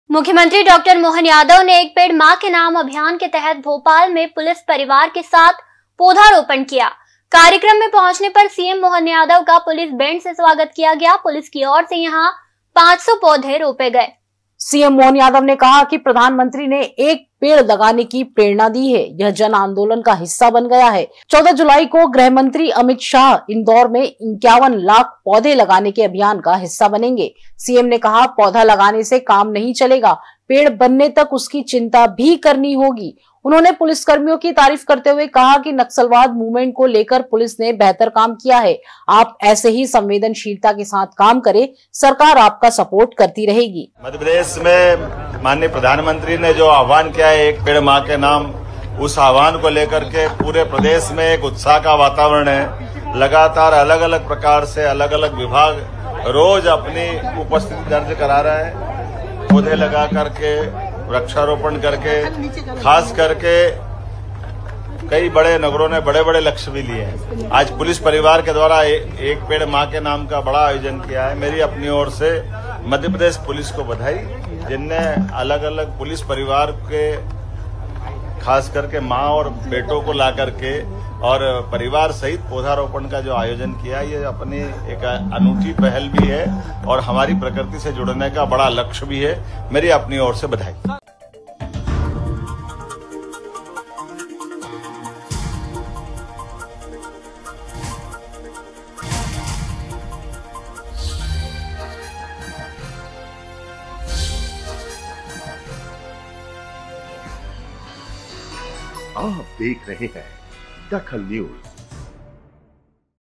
DAKHAL NEWS एक पेड़ मां के नाम अभियान में पहुंचे सीएम
एक पेड़ मां के नाम अभियान में पहुंचे सीएम
पुलिस की ओर से यहां 500 पौधे रोपे गए